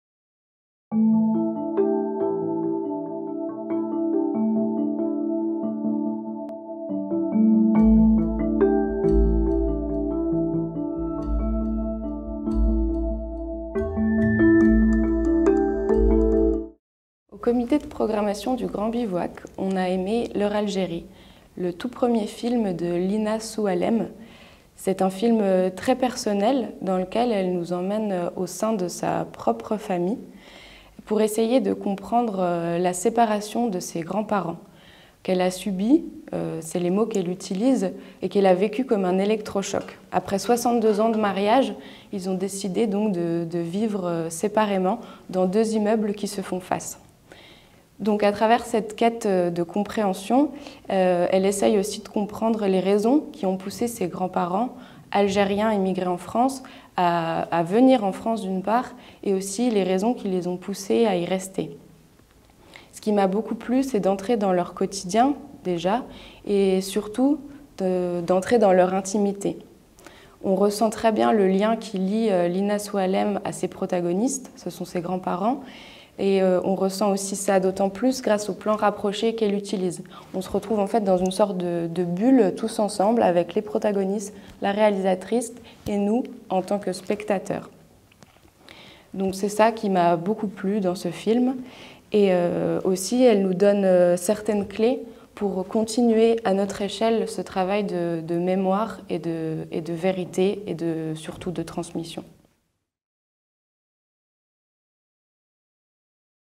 On a aimé Leur Algérie. On vous dit pourquoi. Par Charlotte Perrin, du comité de programmation.
RÉALISATRICE : Lina Soualem • IMAGE : Lina Soualem • SON : Lina Soualem • MUSIQUE ORIGINALE : Julie Tribou, Rémi Durel ­• MONTAGE : Gladys Joujou • PRODUCTION : Agat Films & Cie, Akka Films, Making of Films

L'histoire :
Lui (Mabrouk), sur son arrivée en France il y a plus de 60 ans : « On nous a fait venir pour donner la main. Pas pour gagner plus. Un travail de prisonnier ». Elle (Aïcha), 8 ans à l’époque du massacre de Sétif, évoquant, secouée par le rire, son mariage arrangé à 15 ans : « J’ai rien compris ! ». Lina, leur petite-fille, derrière la caméra, filmant leur vieillesse au repos mais séparée dans deux immeubles face-à-face, à Thiers où leur vie de « Français résidents » a cherché ses racines. Leur fils (Zinedine) le comédien : « On a été élevé dans le mythe du retour ». La petite et la grande Histoire, à quatre voix liées.

« On est tous les deux au bord du ravin... lui un peu plus près ». Et voici Aïcha de nouveau dans le rire et le sanglot. Mais c’est nous qui avons les larmes aux yeux. Le film de Lina Soualem est un hommage sans pareil à sa famille. Et au film-documentaire.